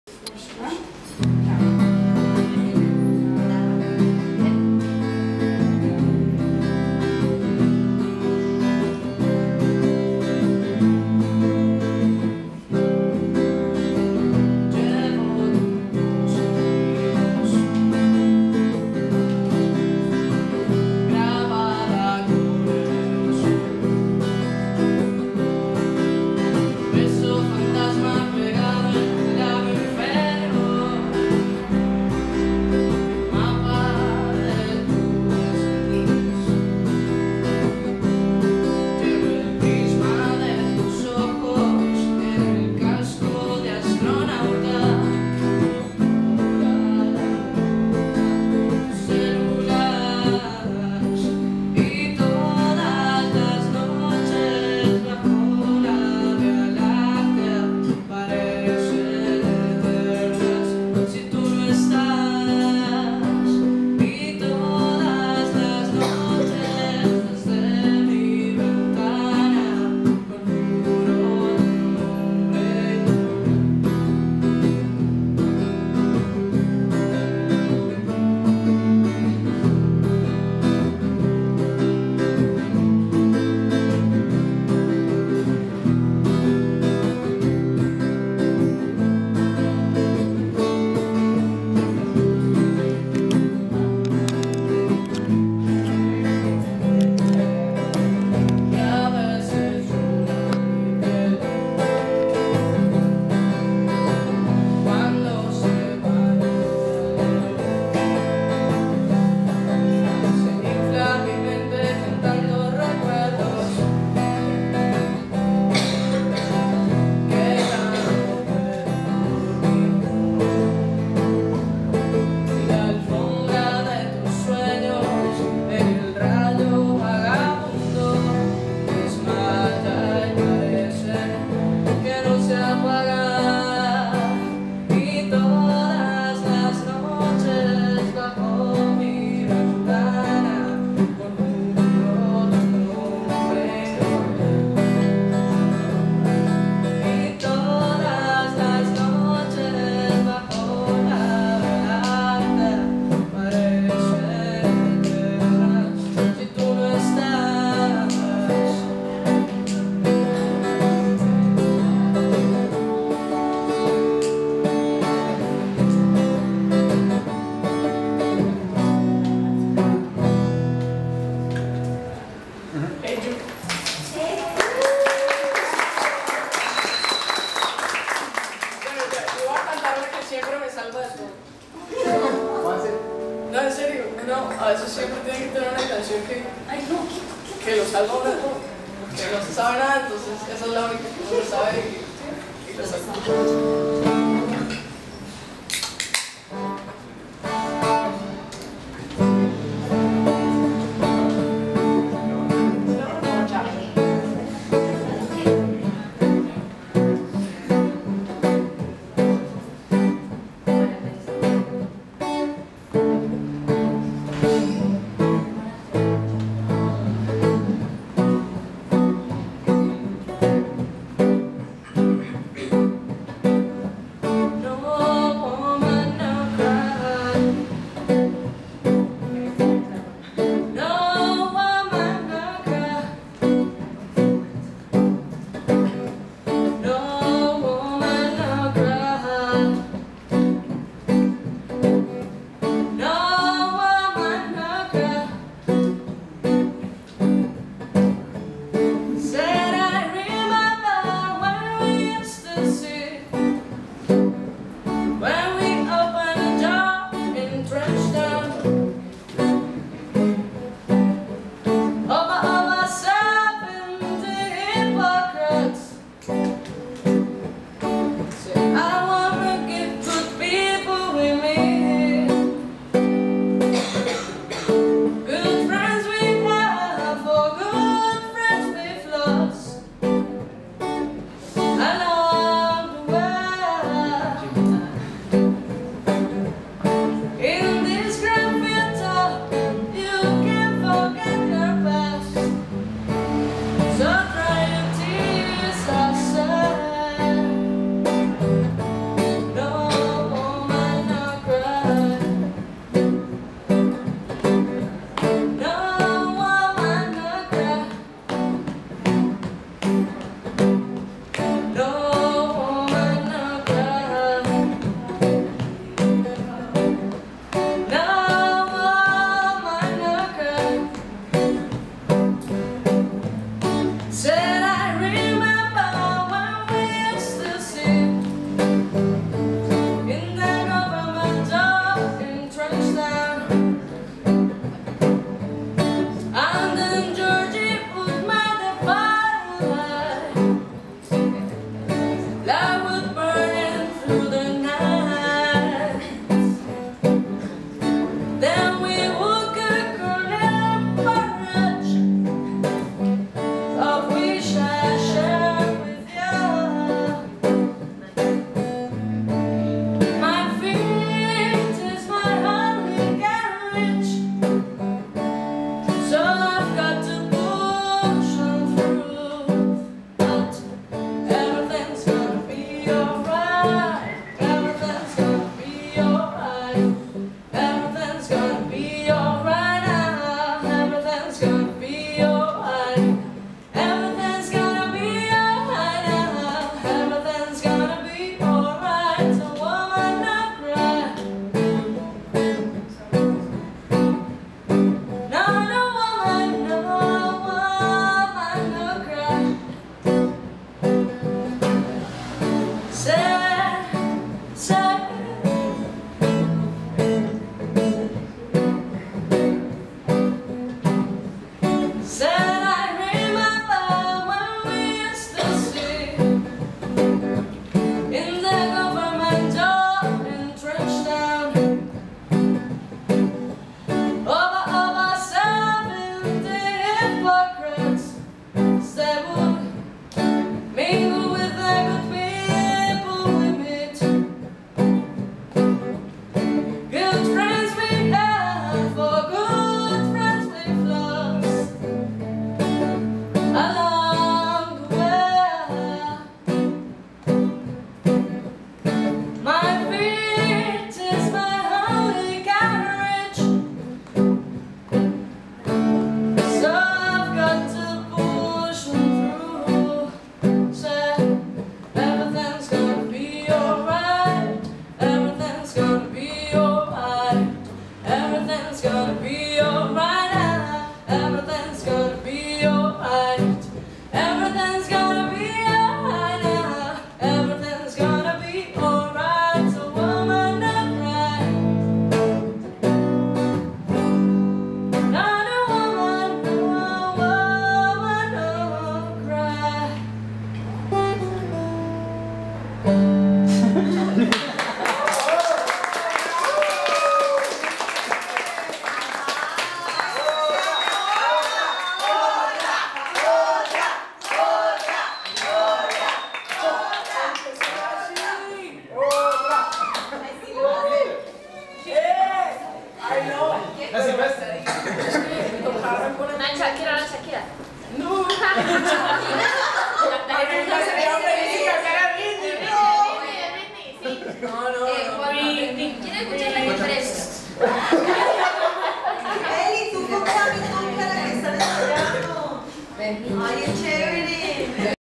Maria P.
28 de Septiembre 2013, Exposición "Universos" de Manuelita Tellez Martínez.
Covers:
- Creo que es una canción de Zoé